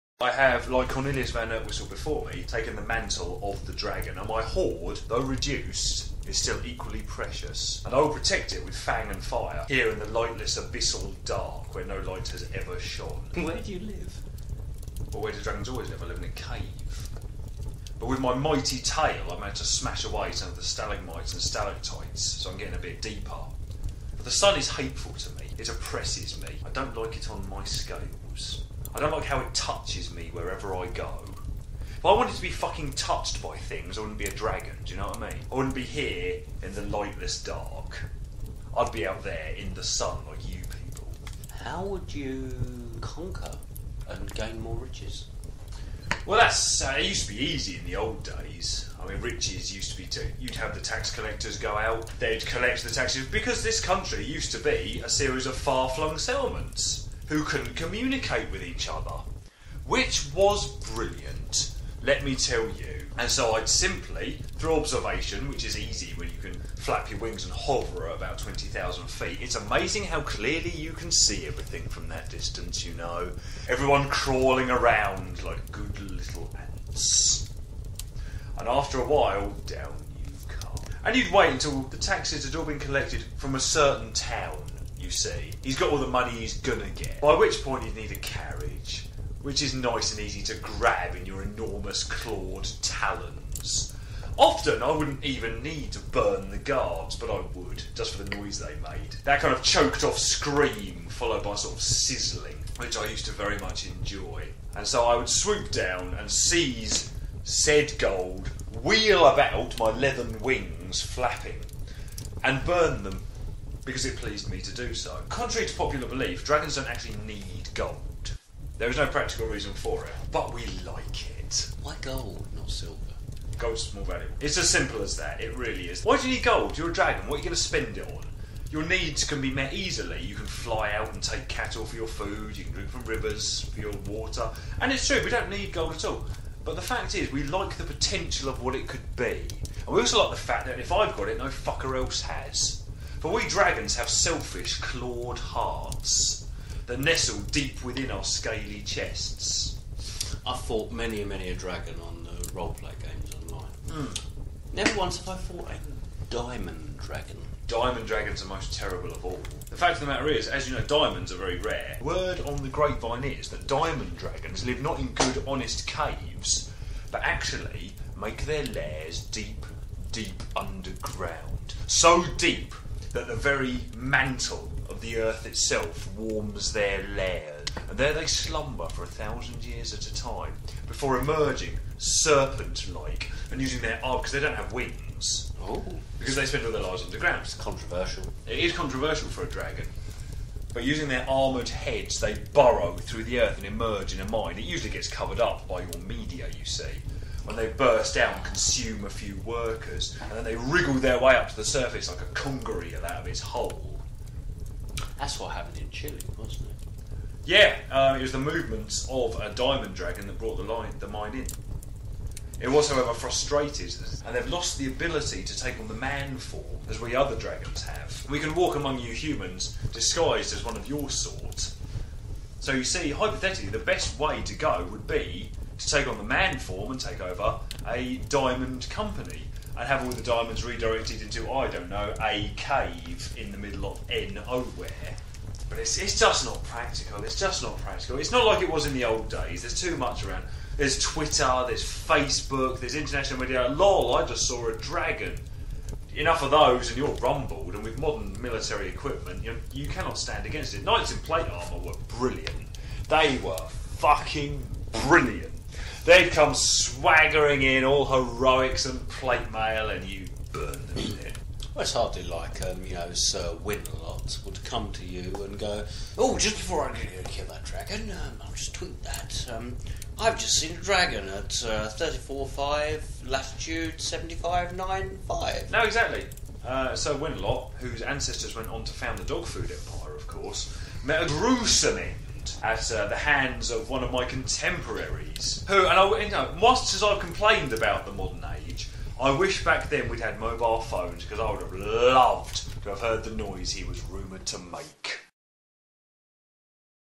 I am a dragon overlord
Max Havoc yet again demonstrates his greatness. This time in a rare insight into his true form. a dragon.